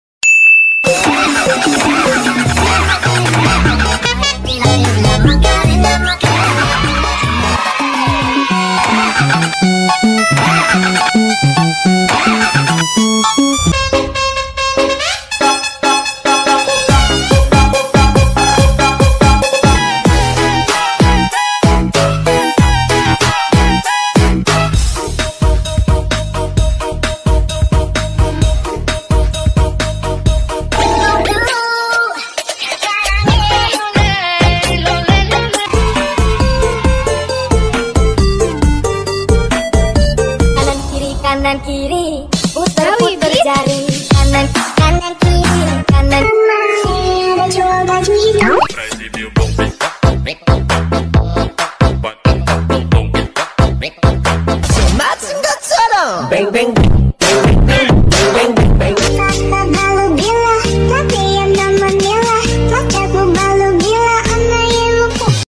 Badut Joget Lucu Among Us Paw Patrol Cocomelon
Kompilasi Badut Joget Lucu Maskot Karakter Among Us Paw Patrol Cocomelon